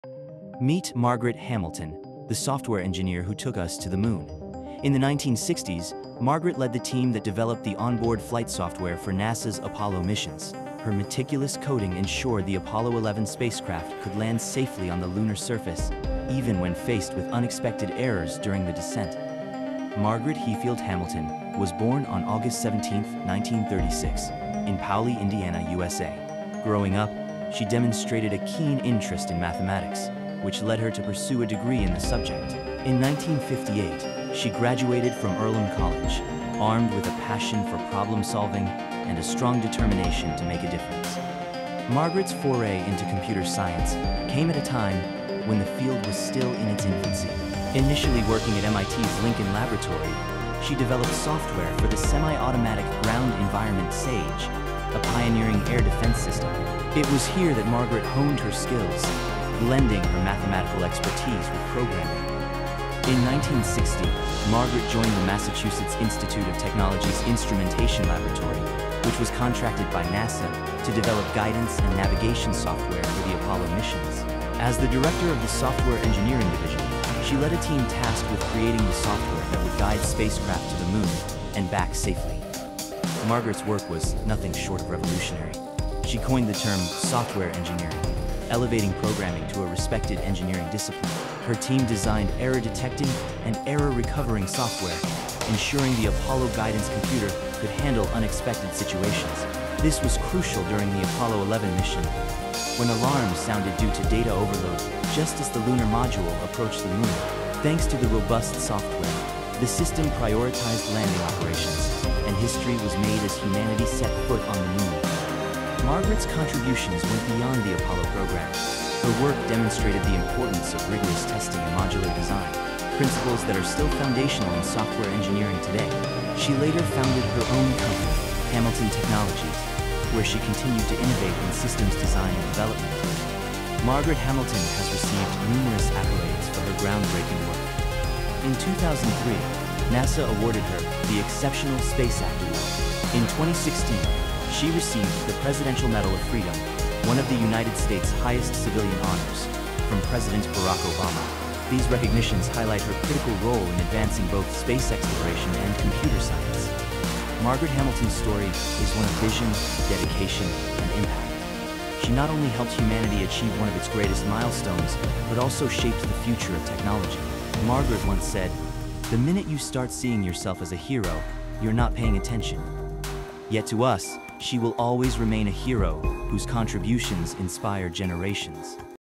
Margaret Hamilton: The Story of the Apollo Software Hero!
Discover the incredible story of Margaret Hamilton, the software engineer who played a crucial role in the Apollo 11 moon landing! 👩‍💻🚀 Her groundbreaking work ensured the safe landing of the lunar module, and her legacy continues to inspire generations.

Following is the Original Apollo 11 Guidance Computer (AGC) source code for the command and lunar modules.